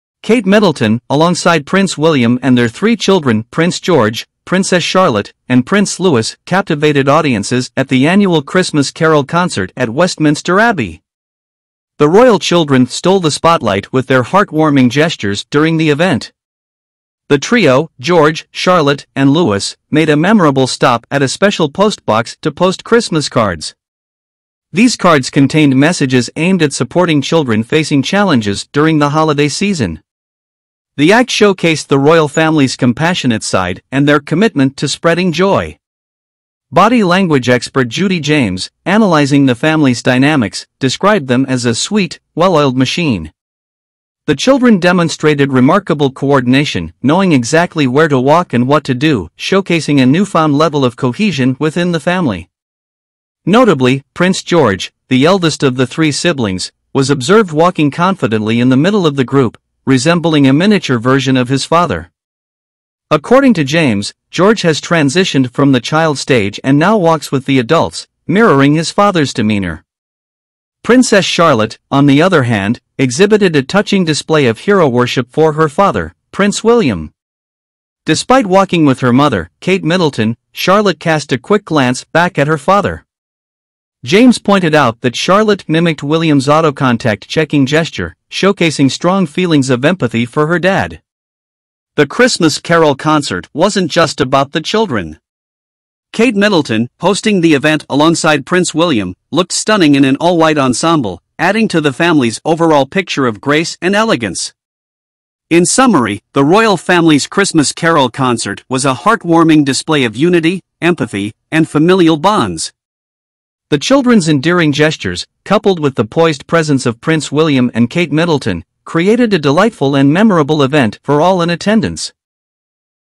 Kate Middleton, Prince William, and their Adorable Children Steal the Show in London! 🎅
Join us as we dive into the magical world of the royal family's enchanting Christmas carol concert, hosted by none other than Princess Kate and Prince William. The event, held at Westminster Abbey, witnessed the delightful presence of the couple's three charming children - Prince George, Princess Charlotte, and Prince Louis.

👑 A Sweet Family Gesture for a Special Cause 👑
The royal trio captured hearts as they paused to post Christmas cards in a dedicated postbox, containing heartfelt messages for children facing challenges this festive season. This heartwarming act was open to all the youngsters attending the event, spreading love and joy during the holiday season.

🤝 A Well-Oiled Family Machine 🤝
According to body language expert Judi James, the family of five showcased a remarkable synchronicity, evolving into a "sweet, well-oiled machine." Their seamless movements and shared understanding were evident as the children confidently walked to the postbox without much prompting, emphasizing a strong family bond.

👶 Prince George's Growing Up Moment 👶
Judi James observed that Prince George has gracefully transitioned from the 'child' stage, now walking confidently with the adults. His demeanor, resembling a mini-me of his father, Prince William, signifies a charming coming-of-age moment for the young prince.

👧 Princess Charlotte's Heartfelt Gesture 👧
The expert noted Princess Charlotte's subtle yet significant gesture, displaying her "hero-worship" for her father, Prince William. Walking with her mother, Princess Charlotte glanced back at her father, mirroring his auto-contact checking gesture. This twinned mimicry suggests a strong sense of empathy and admiration for both parents, especially for her father.

🌟 Princess Kate's Timeless Elegance 🌟
Amidst the family's endearing moments, Princess Kate graced the occasion in an all-white ensemble, looking nothing short of ethereal. Her annual Christmas carol concert at Westminster Abbey, alongside Prince William and their adorable children, was a true spectacle of royal charm.

🎥 Watch the Full Event Unfold! 🎥
Experience the magic of the royal family's Christmas carol concert by watching the full video.